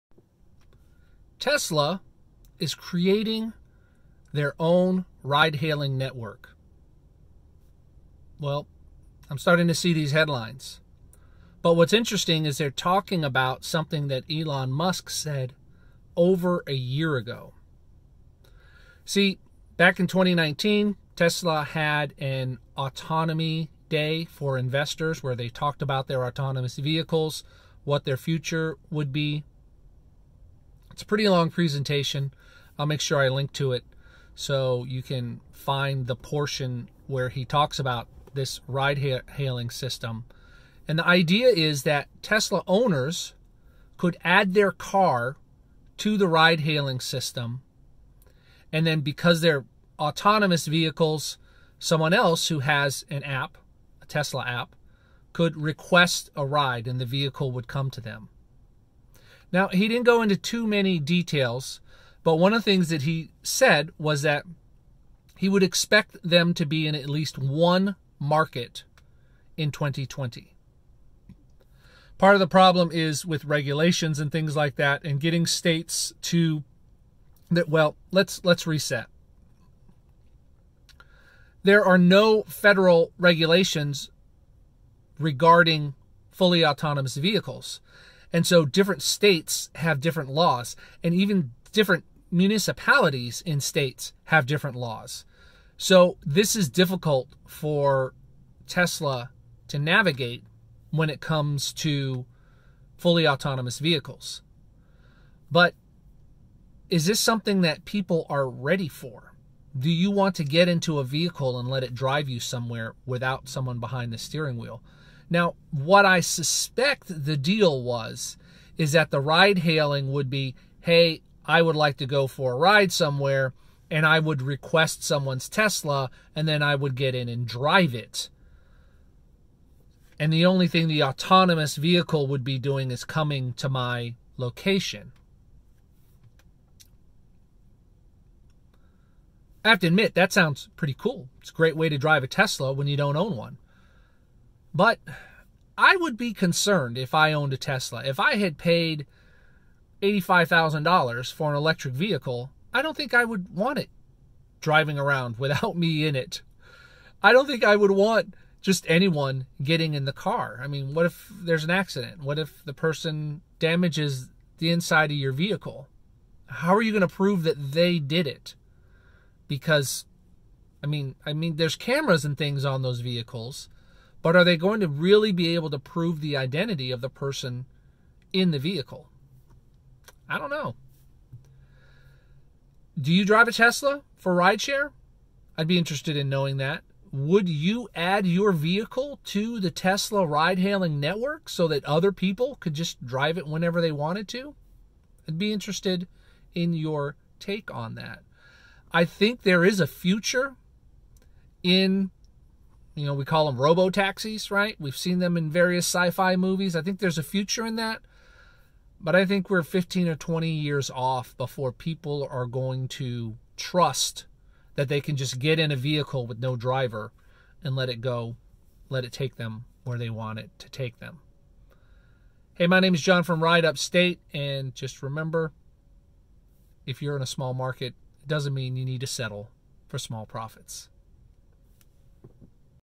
Let's talk about Tesla's autonomous ride-hailing service
Last year, Tesla announced they would have a fully autonomous ride-hailing service available this year. So, where is it?